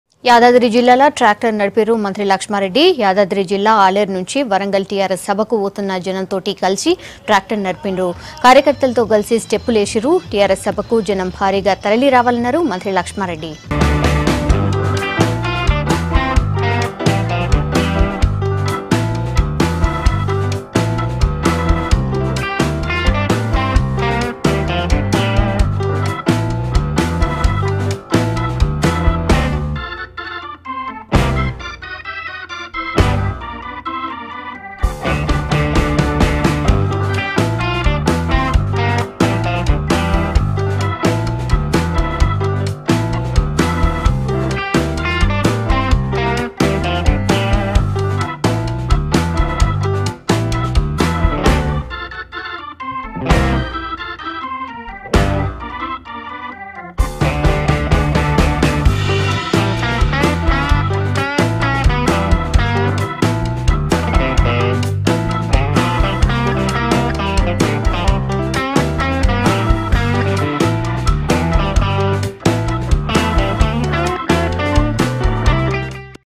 Minister Laxma Reddy Dance With Activists | TRS Pragati Nivedana Sabha | V6 News
Telangana Health Minister Laxma Reddy participated in tractor rally for TRS Pragati Nivedana Sabha in Warangal.